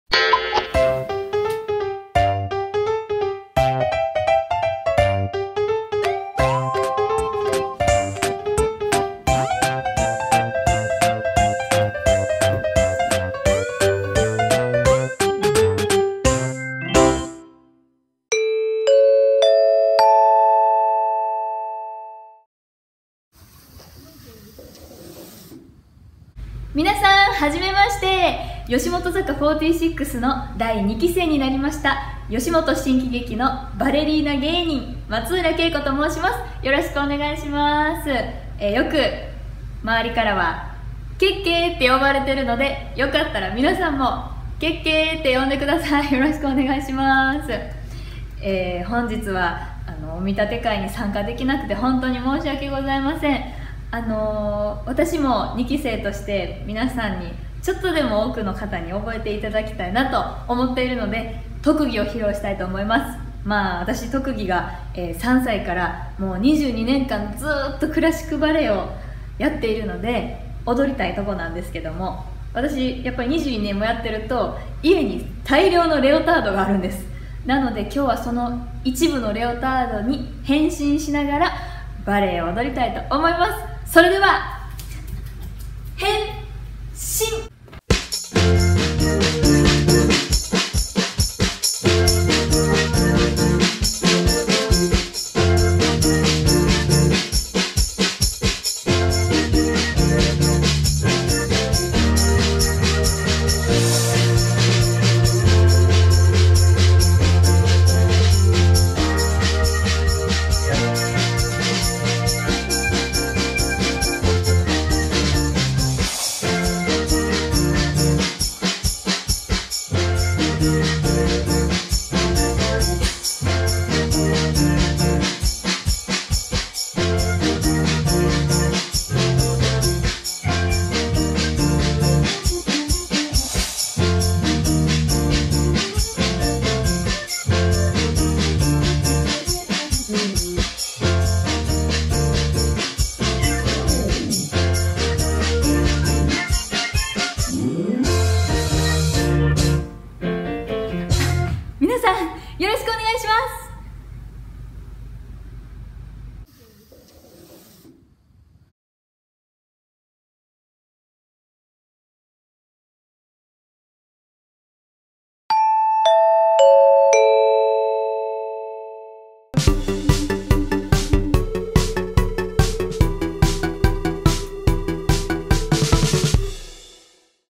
バレリーナ芸人が踊りながらレオタード変身【吉本坂46】お披露目&お見立て会での自己満バレエ（七変化って大変）
この動画を見て、ガチのバレエダメ出しをしてくる人への言い訳を先にしとくね。
→A.床がツルンツルンだったの！許して！

【松浦景子プライベートオンラインレッスン】

【プレゼント💌🎁】
〒542-0075
大阪府大阪市中央区難波千日前11-6 吉本興業 吉本新喜劇 松浦景子 宛